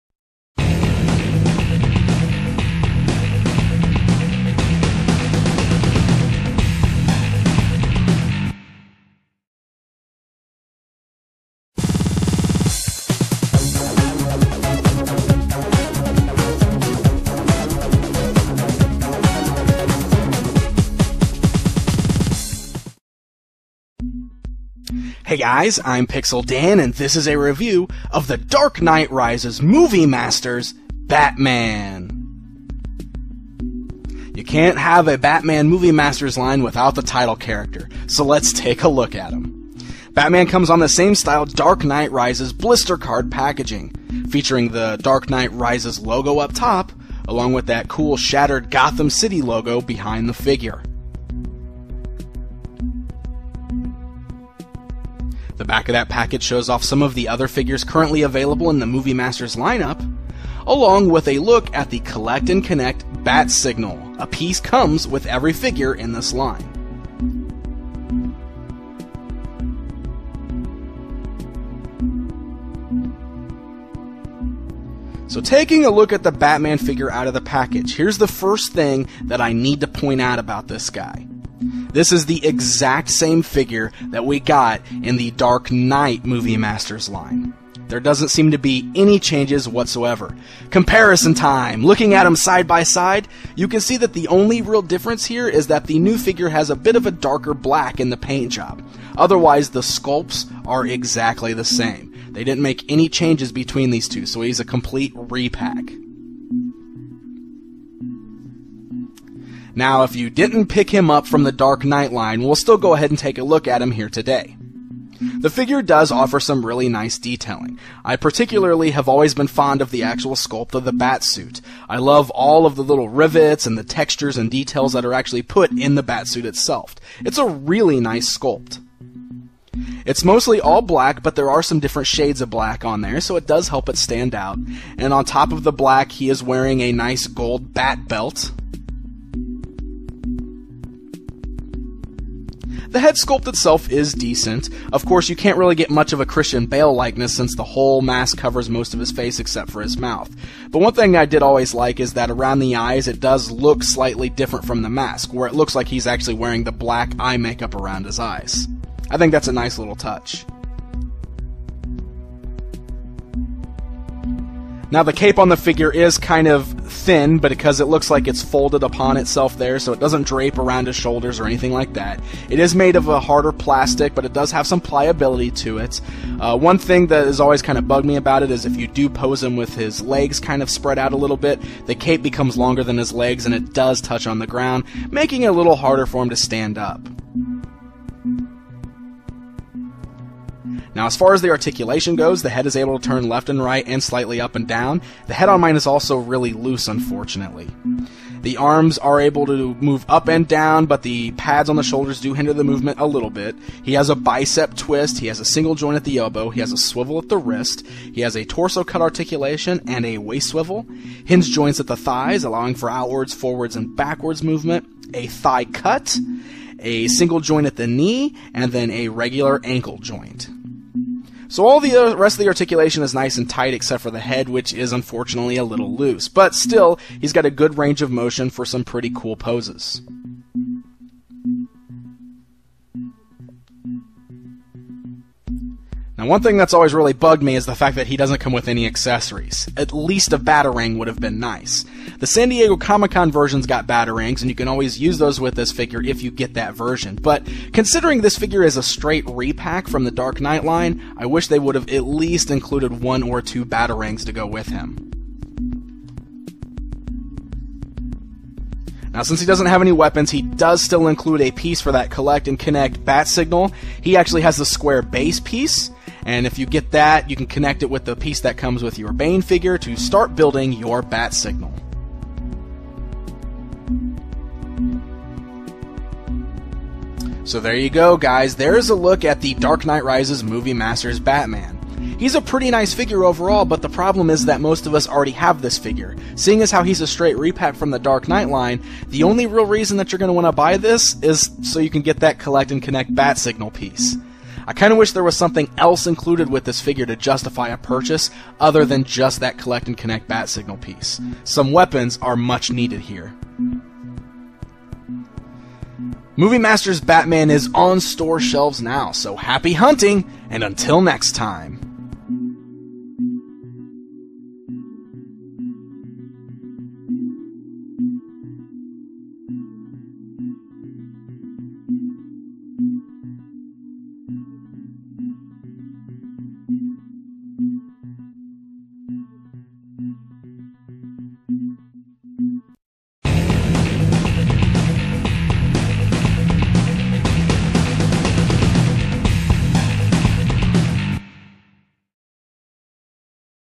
Mattel Dark Knight Rises Movie Masters Batman Figure Review
With the release of Batman: The Dark Knight Rises fast approaching, Mattel has released several new figures in their Movie Masters toy line!

Join Pixel Dan for a look at the film's title character- Batman, from series 1 of the DKR Movie Masters line!